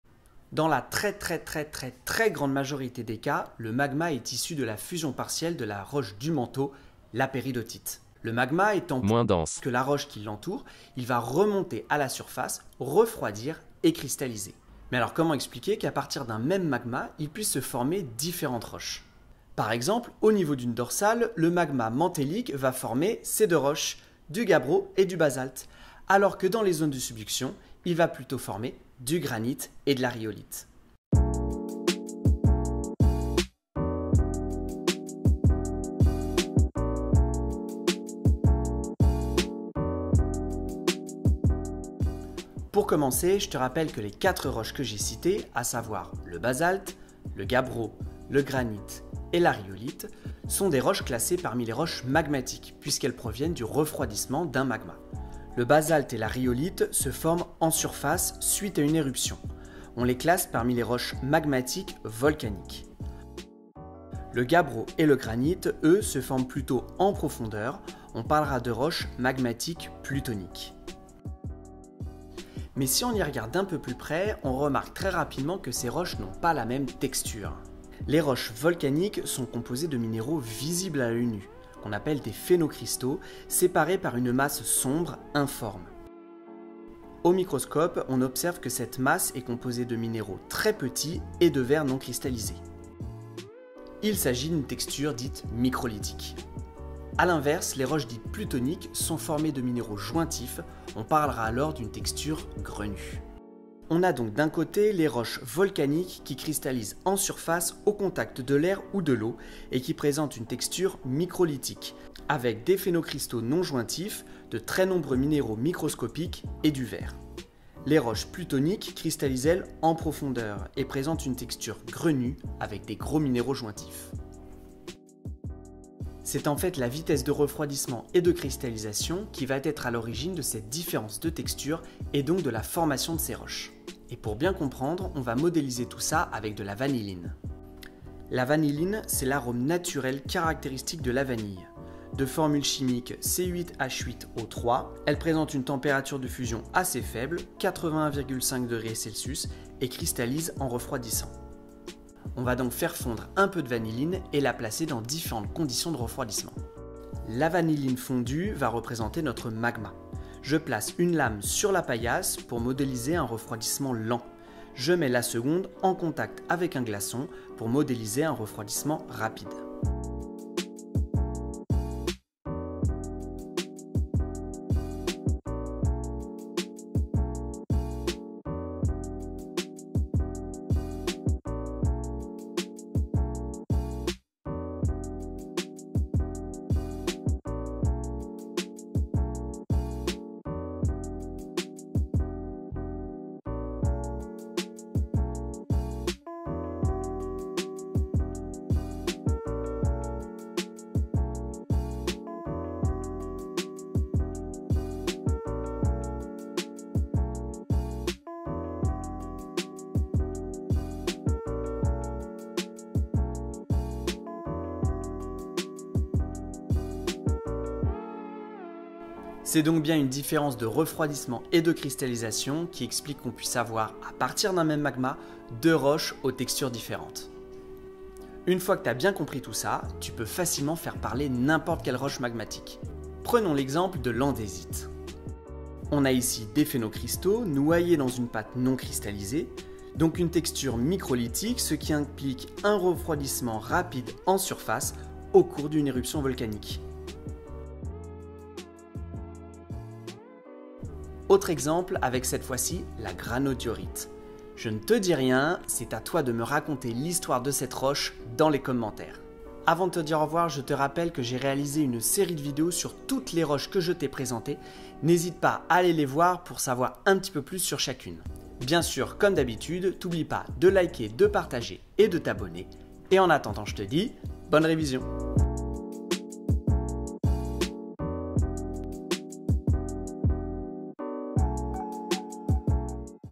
1 magma = 2 roches (Enseignement Scientifique)
Comment à partir d'un même magma il peut se former 2 roches différentes ?

🎥 Vidéos/animations YouTube :  NISHHHDA

✨Gif : justingammon

👂Voix off réalisée avec Voicebooking.com